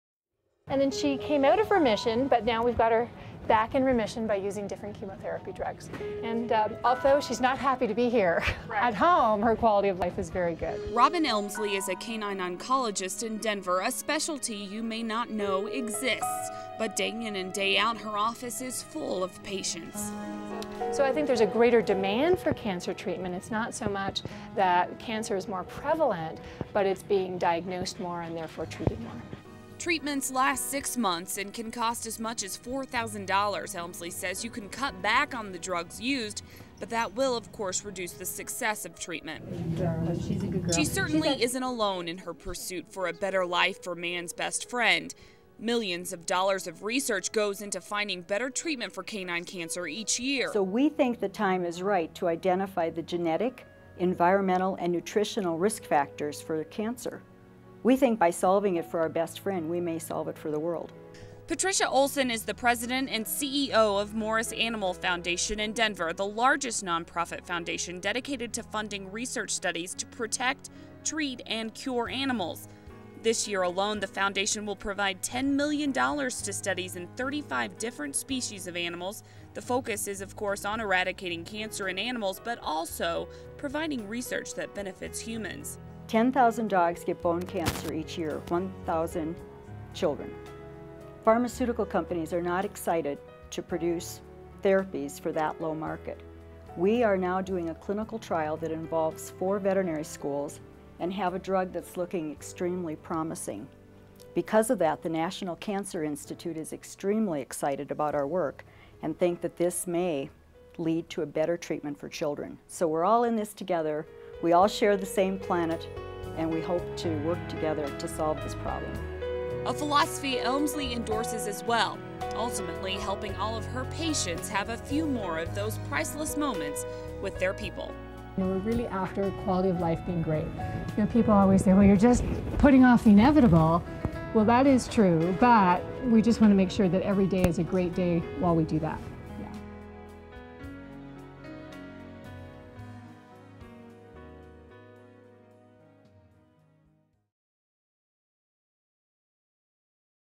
Canine Cancer
A look at a rapid growth of cancer in our pets.